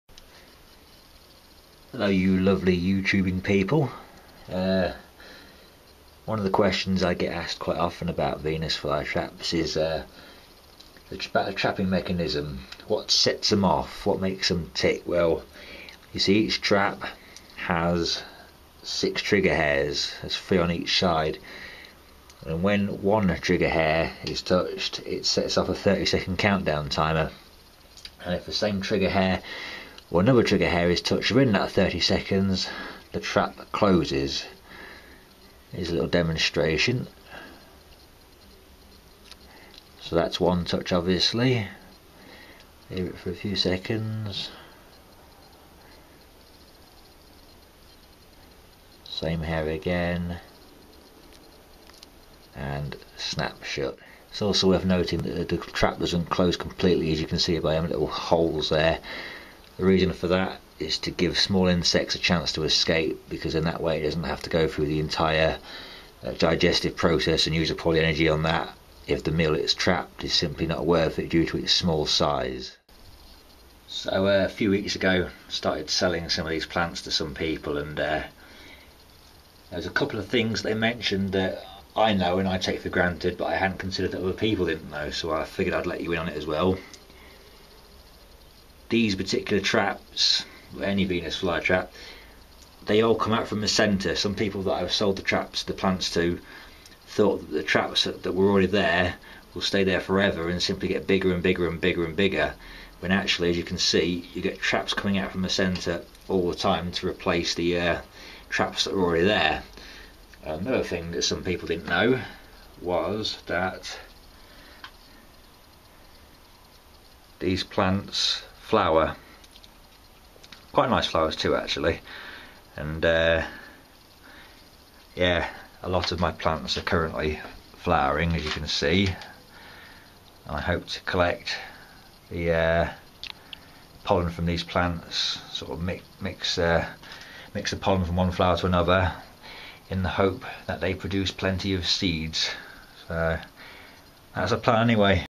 Venus Flytrap Trapping mechanism, trap growing and flowers
Something a little bit different to my normal videos. I demonstrate the trigger mechanism, point out how the traps grow from the centre of the plant, and show its flosers.